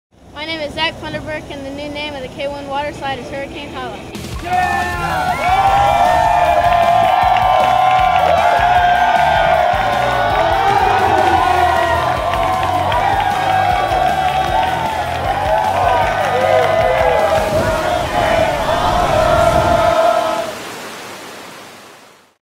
Kanakuk K-1 - The New K-1 Waterslide - 5/30/2010
Zac Funderburk was the winner of the K-1, 'Make a Splash Contest.'  His waterslide name, Hurricane Holla, received the most votes.  Can't wait for you to ride it!